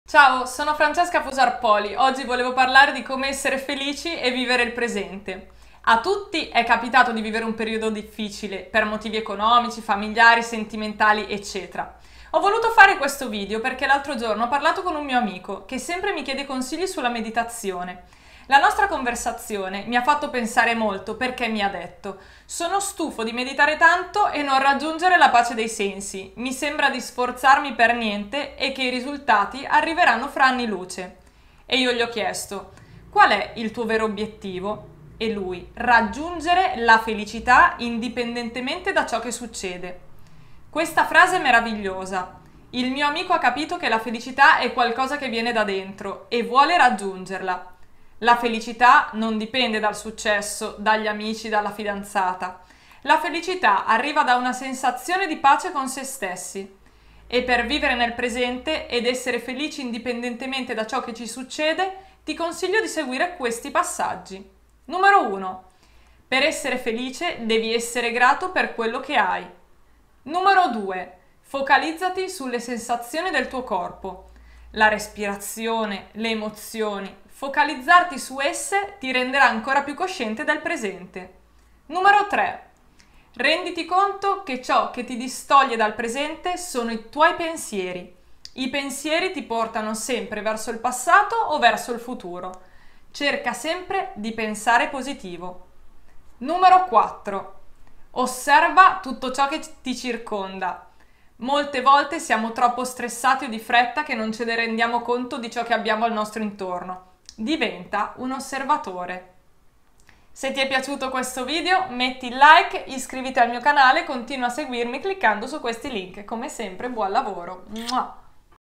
Vivi il presente e sii felice
Ciao sono Francesca Fusar Poli,
oggi parlero' di come vivere il momento presente ed essere felici.

A tutti e' capitato di vivere un periodo difficile.. per motivi economici, famigliari, sentimentali,...

Ho voluto fare questo video perche l'altro giorno ho parlato con un mio amico che sempre mi chiede consigli sulla meditazione
e  la nostra conversazione mi ha fatto molto pensare perche mi ha detto...

“Sono stufo di meditare tanto e non raggiungere la pace dei sensi. Mi sembra di sforzarmi per niente.. e che i risultati arriveranno tra anni luce. “

E io gli ho chiesto .. “Qual e' il tuo vero obiettivo??”

E lui .. "Raggiungere la felicita indipendentemente da cio che succede."

Come sempre buon lavoro e grazie per seguirmi!

Francesca FP